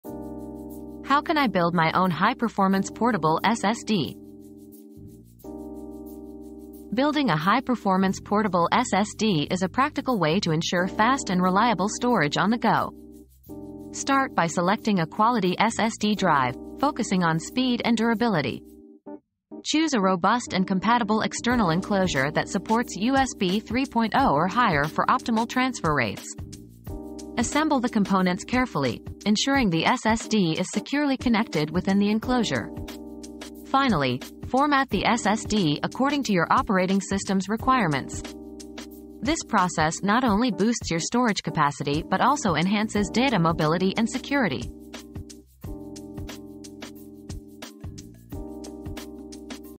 How Can I Build My Own High-Performance Portable SSD?
Build Your Own High-Performance Portable SSD • DIY Portable SSD • Learn how to build your own high-performance portable SSD for fast and reliable storage on the go. Find out the essential components and steps to ensure optimal transfer rates, data mobility, and security.